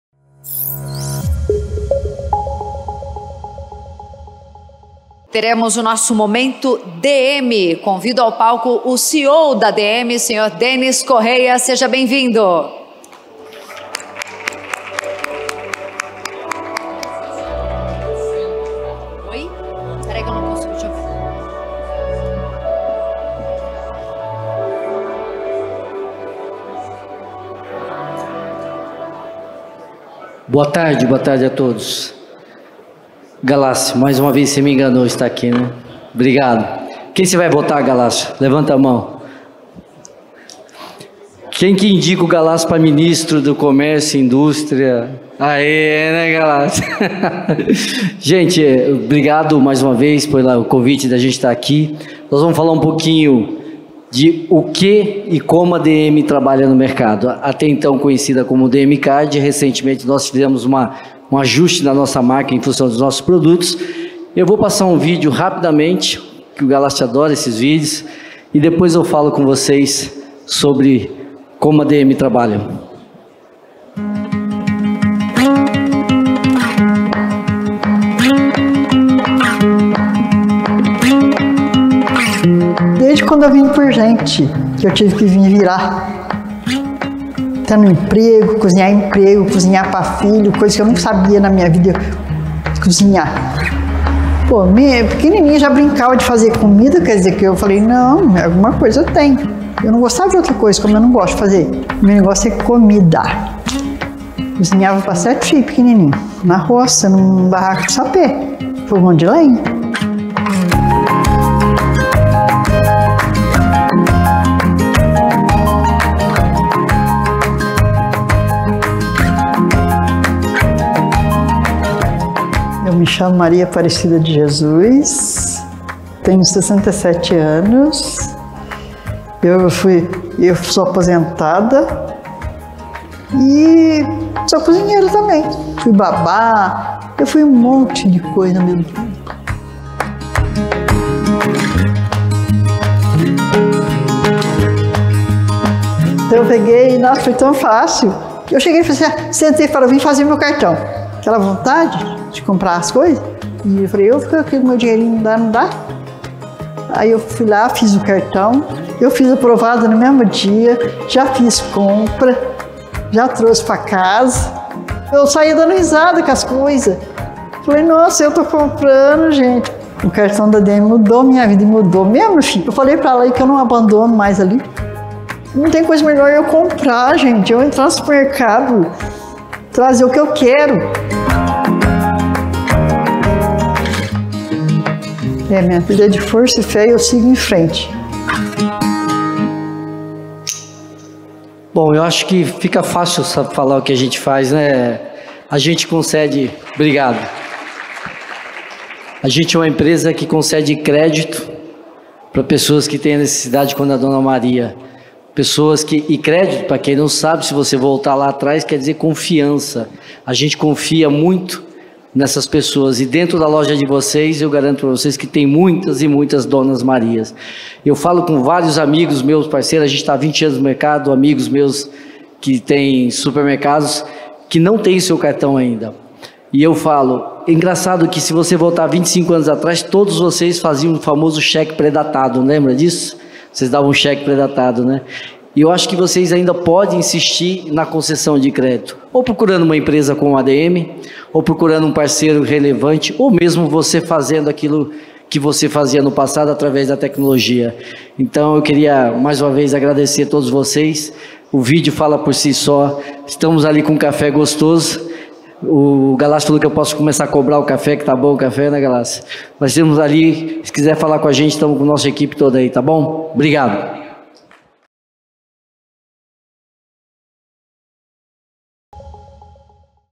Momento DM
Confira o que rolou na 56ª edição da Convenção ABRAS, maior encontro de lideranças do setor supermercadista, que neste ano teve como tema A Nova Economia Supermercadista.